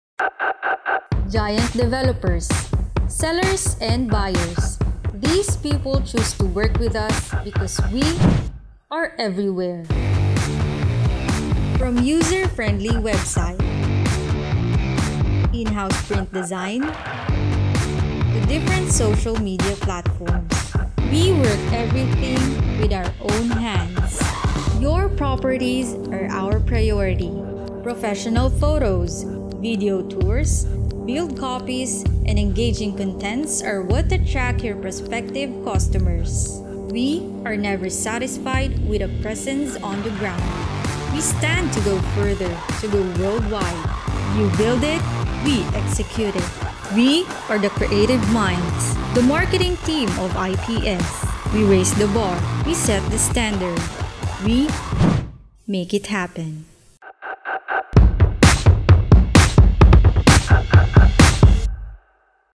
IPS TV Episode 05: Cambodia Real Estate | Introducing IPS Marketing Team - We Make It Happen!
IPS-Cambodia is known as a real estate agency, but here's to reveal that we aren't. We are a creative team that markets real estate through impactful branding, global presence, and multimedia capabilities.

today and browse over 5,000 properties for rent and sale!